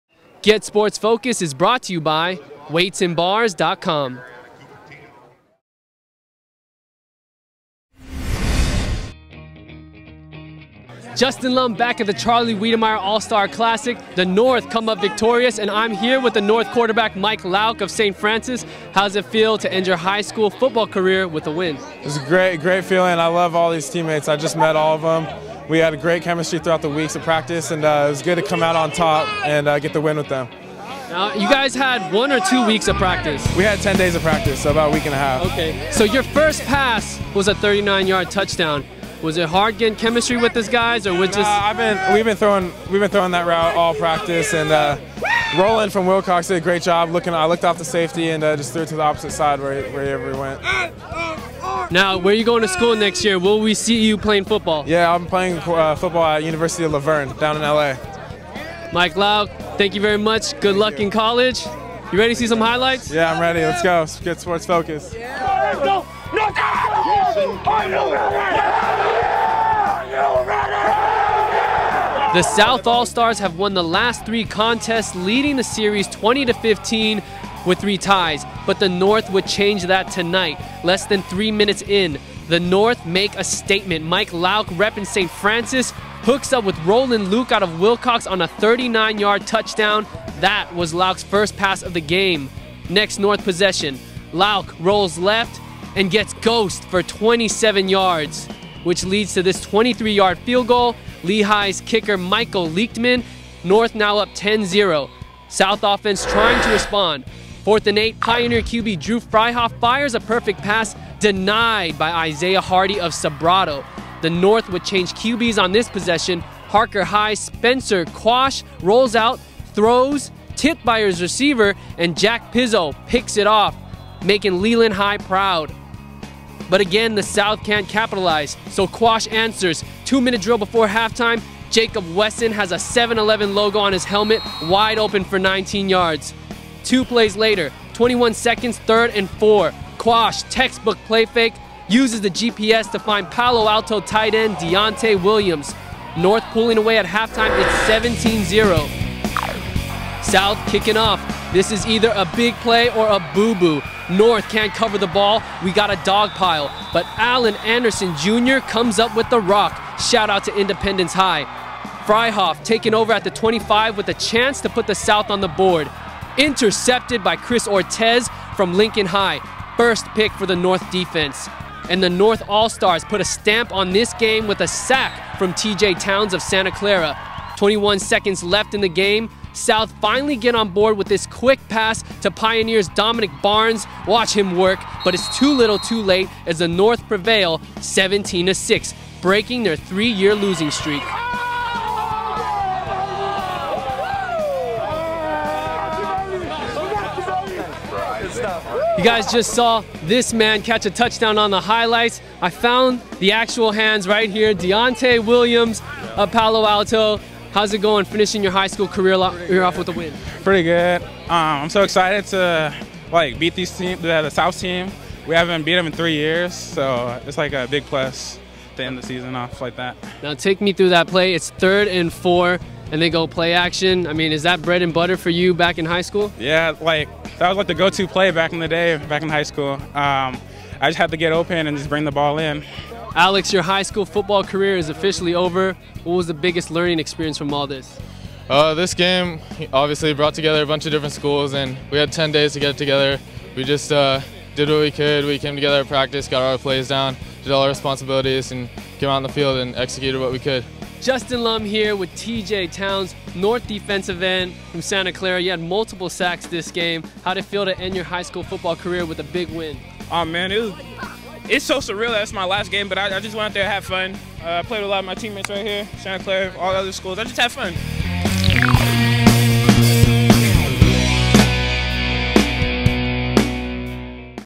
39th Annual Charlie Wedemeyer All Star Game - GSF Weekly w/ Justin Lum
GetSportsFocus.com - The 39th Annual Charlie Wedemeyer All-Star Game was the celebration of many different high school football careers. The South team had won the last three contests and led the series with a 20-15-3 record against the North team. That streak was broken in the 2013 matchup as the North won 17-6.

North All-Star quarterback Michael Lauck of St. Francis started things off on his first pass, throwing a 39-yard touchdown to Roland Luke of Wilcox. On the next North possession, Michael Leitchman would kick a 23-yard field goal making it a 10-0 score. Harker High's Spencer Quash would bounce back from an interception and lead the North to another score, throwing a 4-yard touchdown to Palo Alto's De'Antay Williams. The score would be 17-0 at halftime as the North All-Stars pulled away.

Offensive drives sputtered for the South most of the game until Santa Teresa quarterback Austin Richelle led a last minute touchdown drive, hitting Dominic Barnes of Pioneer on a quick pass but it was too late. The South still lead the Charlie Wedemeyer All-Star series 20-16.

Special Thanks to:
Silicon Valley Youth Classic
SJSU Athletics

If you have any events you would like us to consider going to or feature story ideas... anything sports related, we'll try our best to get you covered.

CREDITS
produced by SportsFocus Production
written by: Justin Lum
staff intern: Edwin Wise
video by: Joey De Stefano, ALF Joaquin
editor: ALF Joaquin